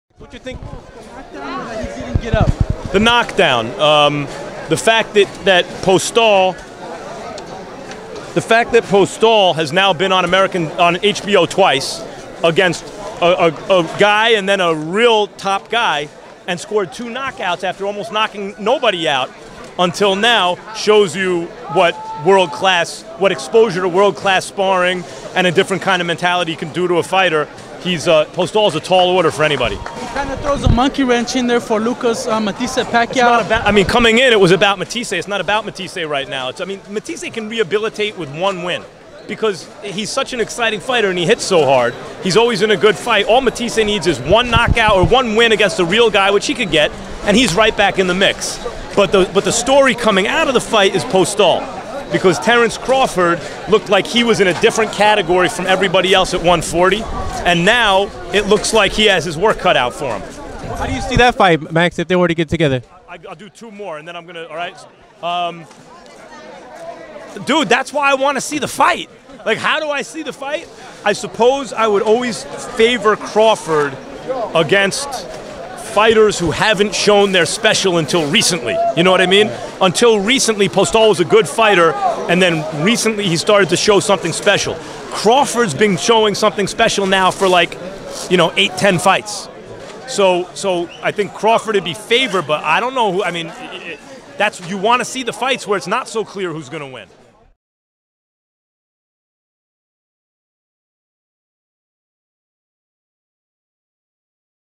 MAX KELLERMAN REACTS TO VIKTOR POSTOL KO OF LUCAS MATTHYSSE; TALKS POTENTIAL TERENCE CRAWFORD CLASH
"Postol is a tall order for anybody...Terence Crawford looked like he was in a differenct category from everybody else at 140 and now, it looks like he has his work cut out for him," stated HBO commentator Max Kellerman, who shared his thoughts on WBC jr. welterweight champion Viktor Postol's stunning KO of Lucas Matthysse and discusses a potential clash with WBO jr. welterweight champion Terence Crawford. Check it out!